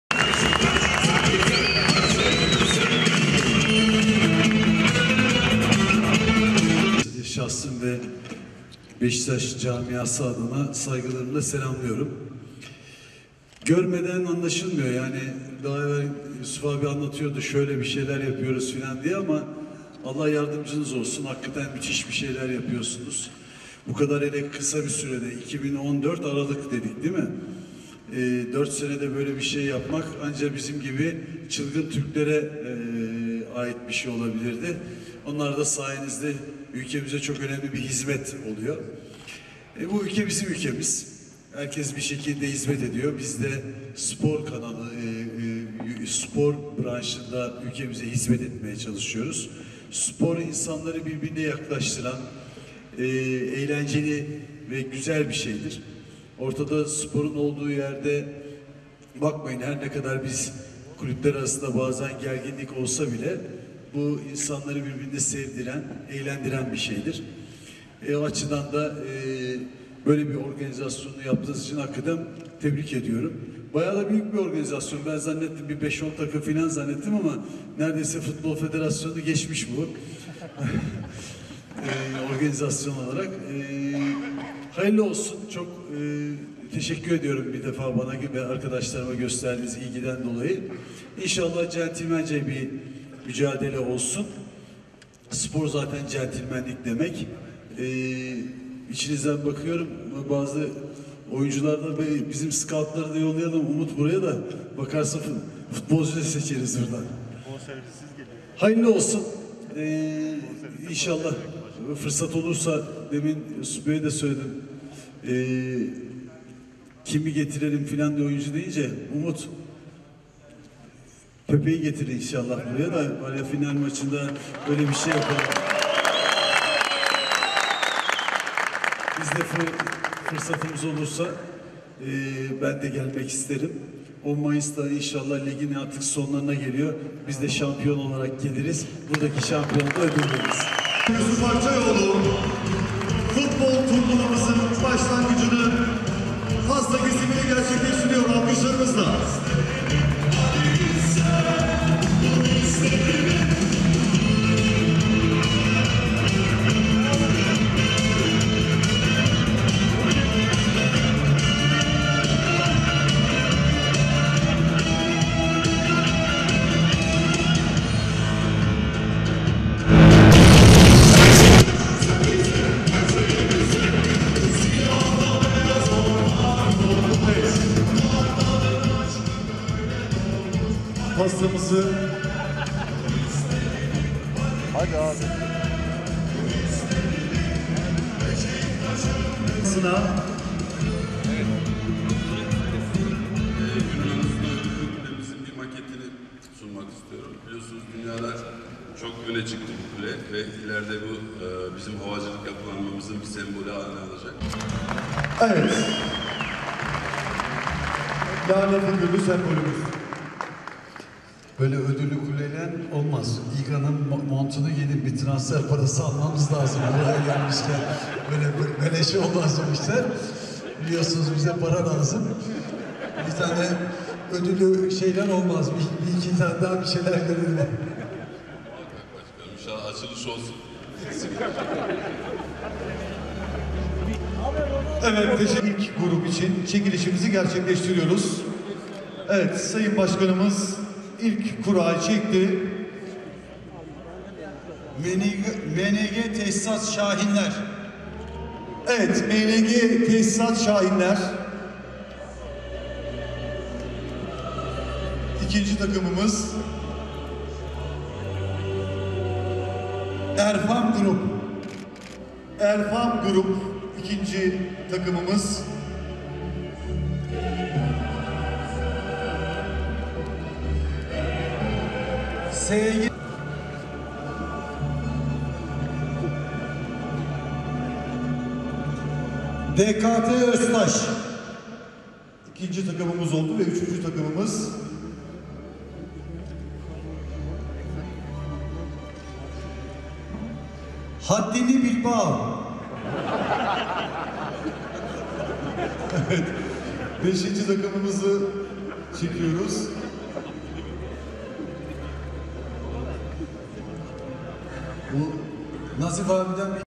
Fikret Orman, FutbolİGA turnuvasının kura çekimine katıldı
İstanbul Yeni Havalimanı çalışanlarının mücadele edeceği FutbolİGA turnuvasının kura çekimini Beşiktaş Başkanı Fikret Orman gerçekleştirdi.